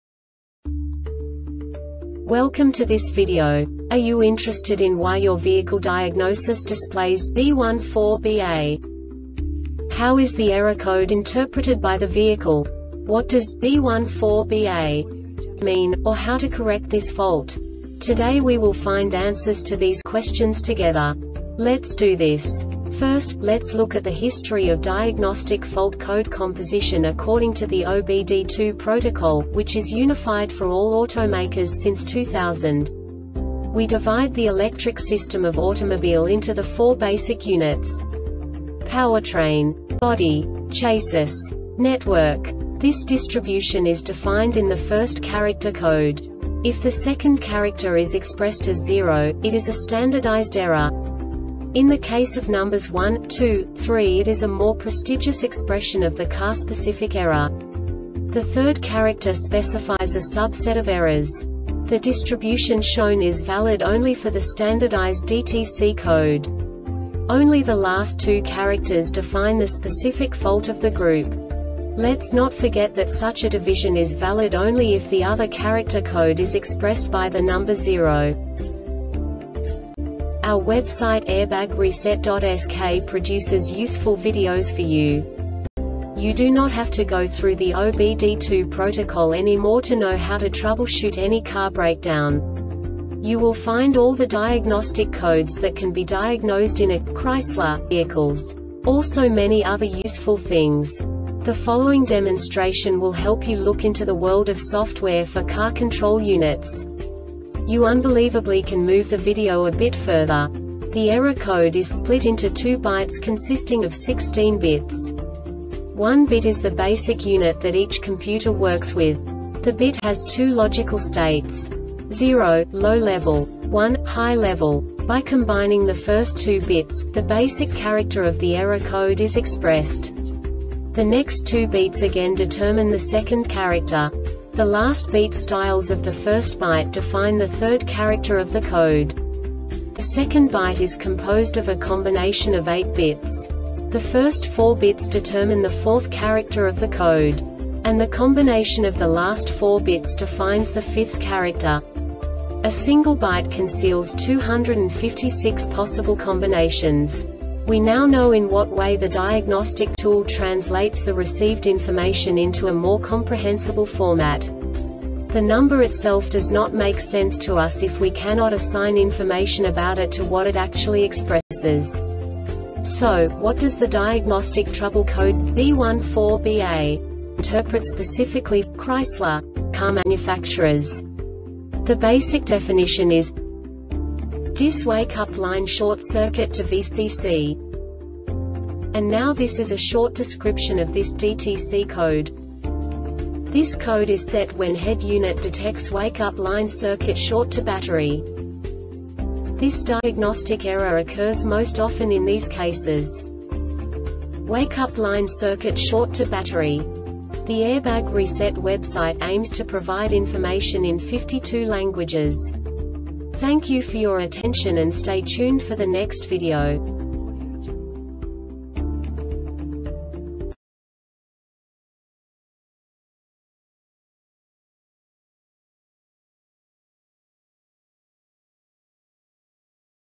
DTC Chrysler B14BA Short Explanation
Contents:
0:21 Basic DTC analysis according to OBD2 protocol standard.
1:48 Insight into programming
2:58 A diagnostically understandable description of the Diagnostic Trouble Code
3:05 Basic error definition

Make:
Chrysler
Code:
B14BA
Definition:
DIS Wakeup Line Short Circuit To Vcc
Description:
This code is set when Head unit detects wake up line circuit short to battery.
Cause:
 Wakeup Line circuit short to battery